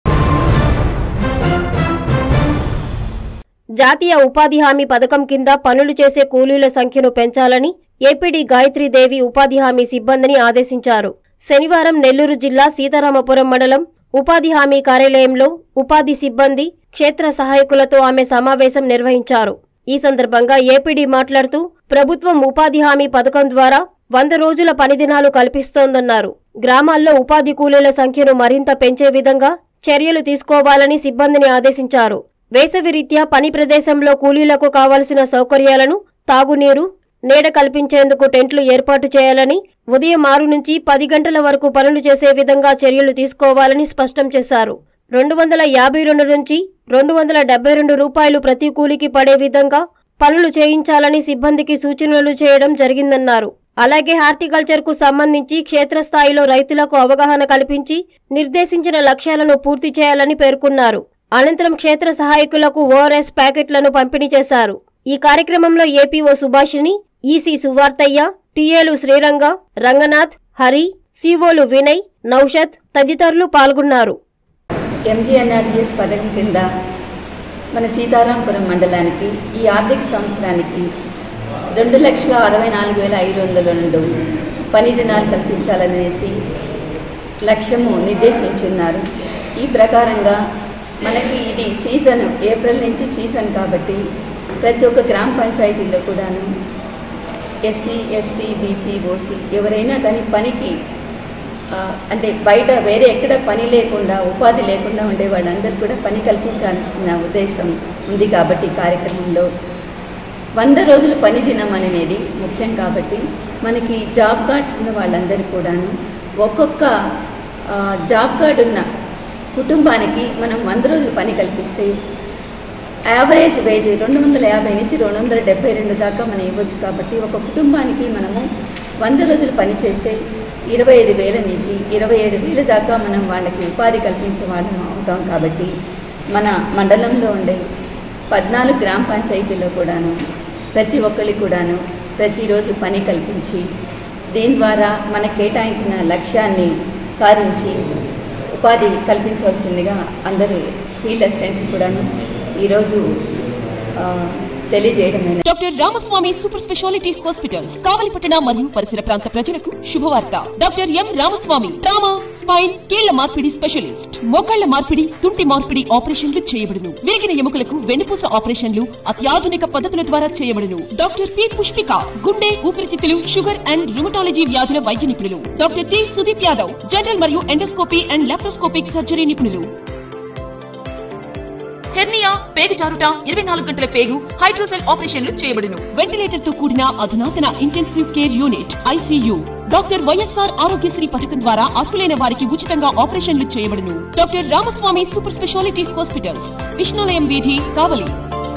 ఉపాధి కూలీల సంఖ్య పెంచాలి...
- ఏపీడీ గాయ‌త్రీదేవి
- సీతారామ‌పురం ఉపాధి హామీ కార్యాల‌యంలో స‌మావేశం